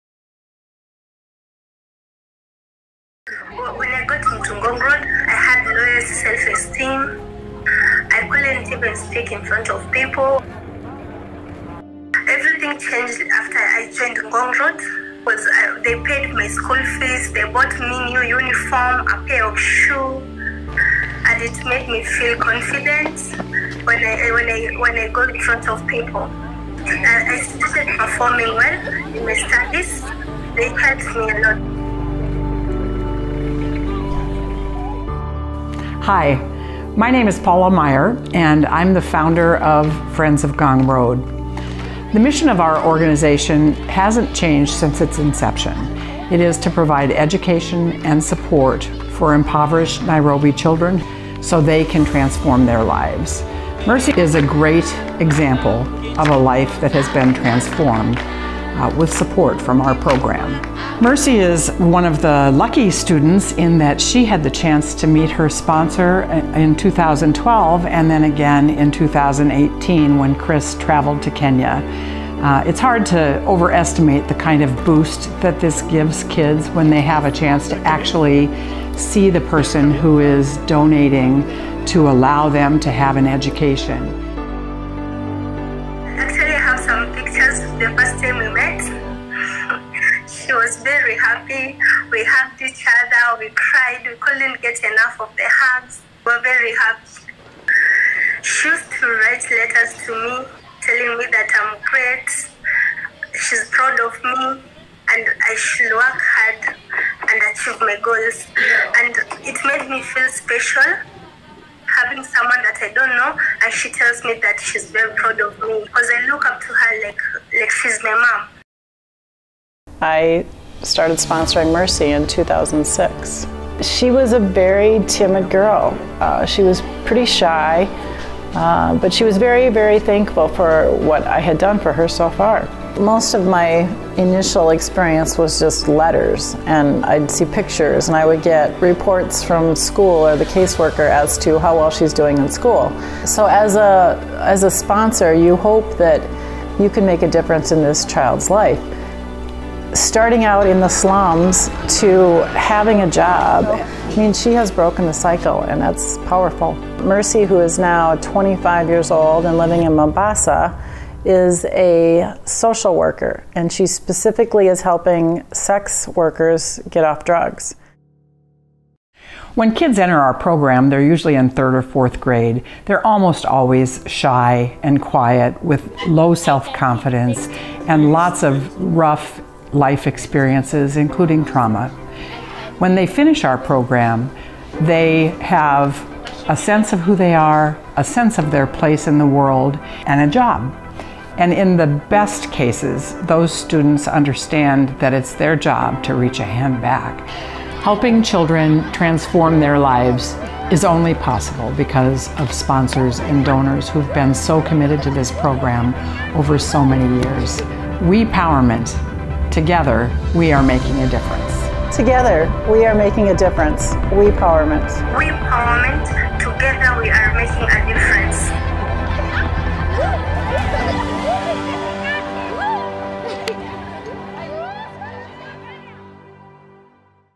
WePowerement V2
Each child generally comes to us from unimaginable circumstances. In their short life, at a young age, they have already experienced severe illness among family members, loss of parents and family and friends, turbulent households, and the need to exist in small crowded homes lacking plumbing for water or sanitation, and where electricity to power lights is a luxury. More than anything they want to attend school. Understandably, Friends of Ngong Road children arrive lacking confidence and sense of oneself.

Through the acceptance, commitment, and caring guidance, Friends of Ngong Road children gain access to education build a sense of empowerment. Better yet, they transform and attain a sense of wepowerment

We coined the word wepowerment because empowerment in and of itself falls short of describing the transformation. Importantly, gaining empowerment critical, transitioning children from a minimal sense of self towards autonomy and self-determination. Wepowerment. however, goes beyond the individual themself. Wepowerment flips the "m” in empowerment to a “w”, as in We recognizes that each and all of us are connected, all a part of a whole, and that not one of us lives autonomously. We get to where we are by the help of others.

Students of Friends of Ngong Road experience that. Their lives are transcended to live independently and as a part of a whole. They are growing up and giving back to the community that helped get them to where they are today.